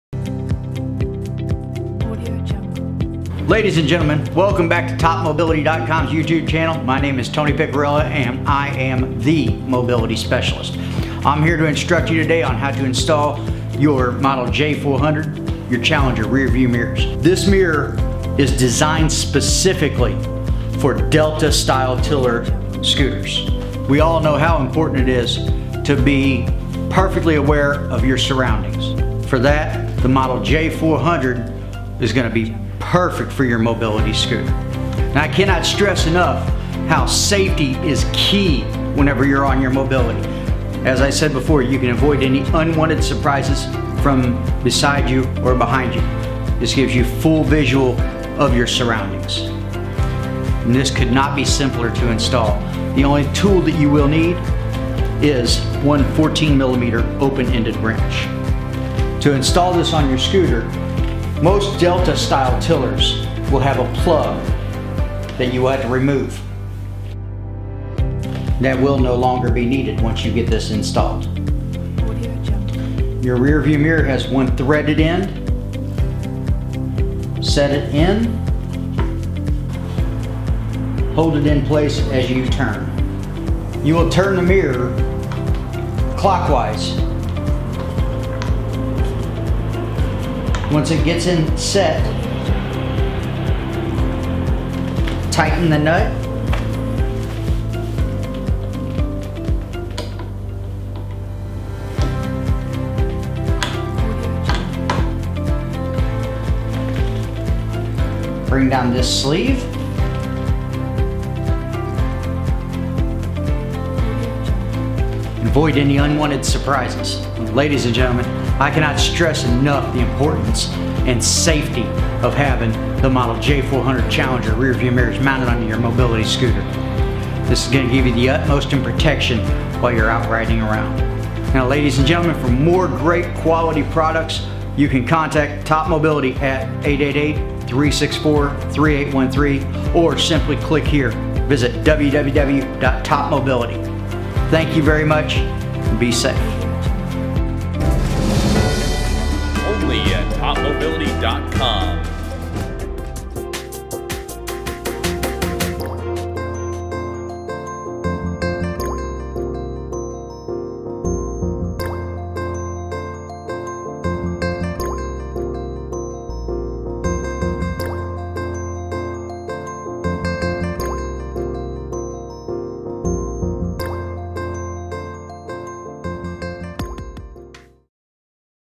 Challenger Mobility Deluxe Scooter Rear View Mirrors J400 Delta Tiller Pride, Golden, Drive
The Challenger Mobility Deluxe Scooter Rear View Mirrors Assembly - (1) Pair model# J400 Compatible with the Go-Go Sport, Go-Go LX with CTS, Go-Go Elite Plus, Plus HD, Victory 9, Victory 10, Celebrity X 3 and 4 wheel, and Pursuit Scooters.
Compatible with Drive mobility scooters models: Osprey, Pilot, and Prowler series
Compatible with the Challenger Sport and Challenger XL scooters.
Compatible with most new generation Mobility Scooters with screw holes and delta tiller.
Set of 2: 1 left and 1 right hand side Mount.
Screws into the handlebar.